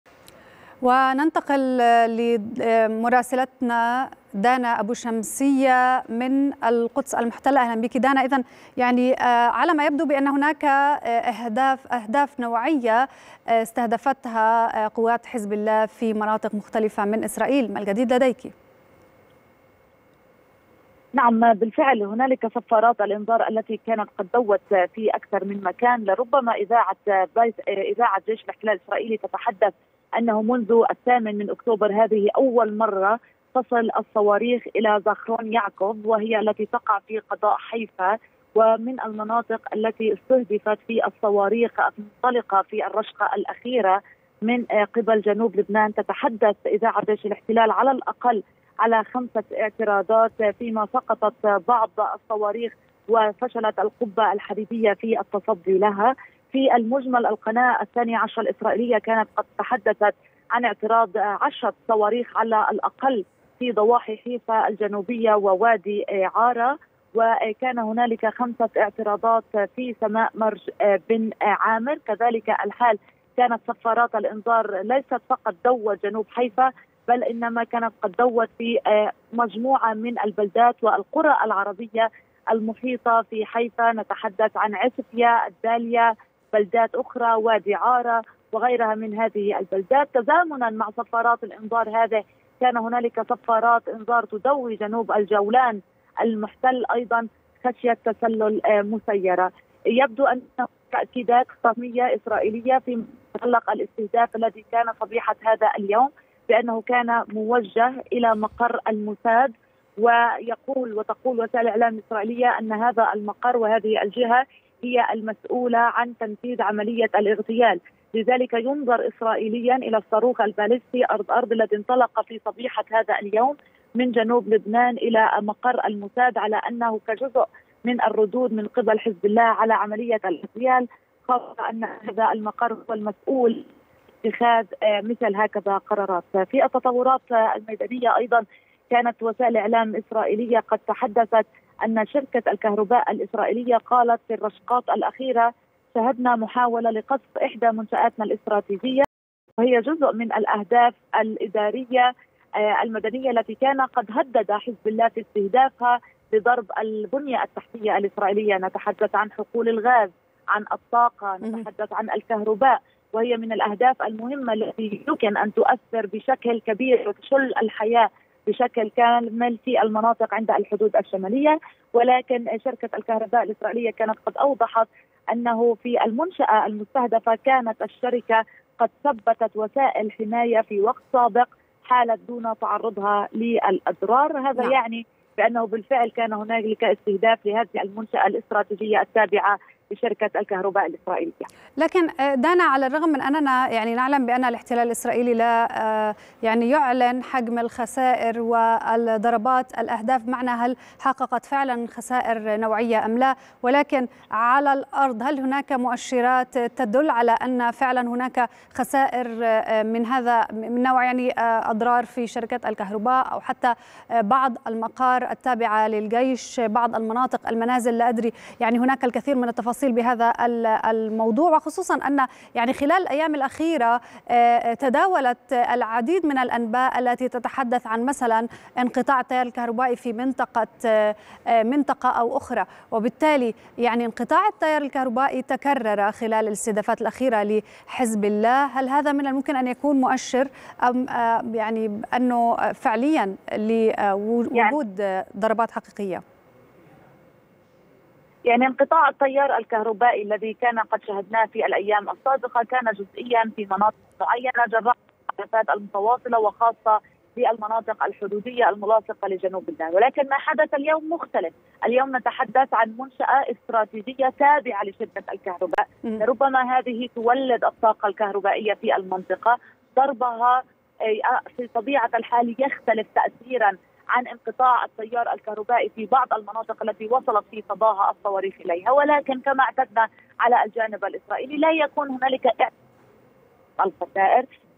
مراسلتنا: إعلام إسرائيلي يتحدث عن اعتراض 10 صواريخ في ضواحي حيفا الجنوبية
25/9/2024